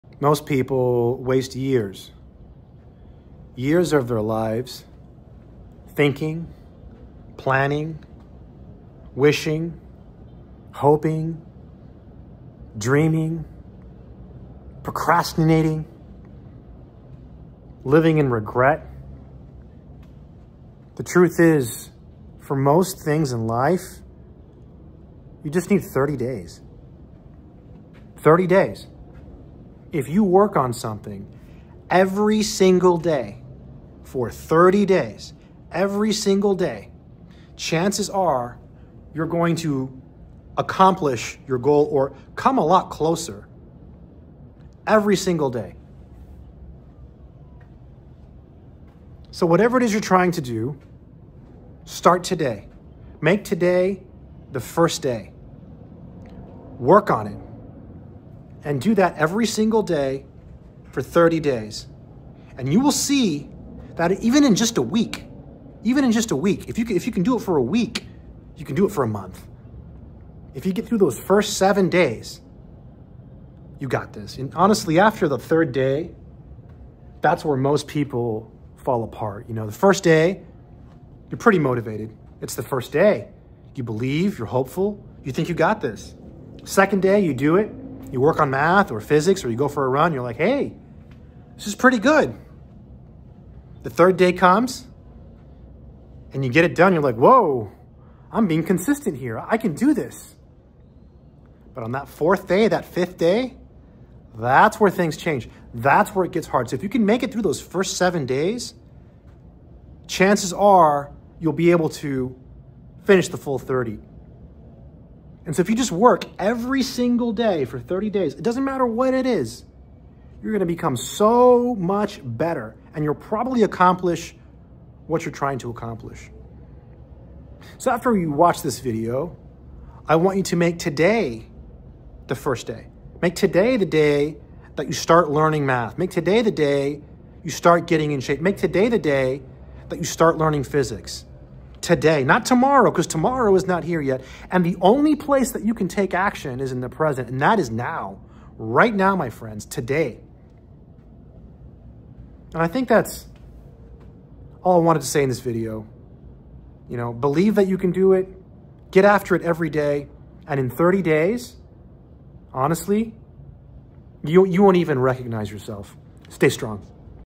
STOP WASTING YEARS. 30 DAYS IS ALL IT TAKES.
Most people waste years and never reach their goals, but in reality, you can do a lot in 30 days. Stay strong my friends.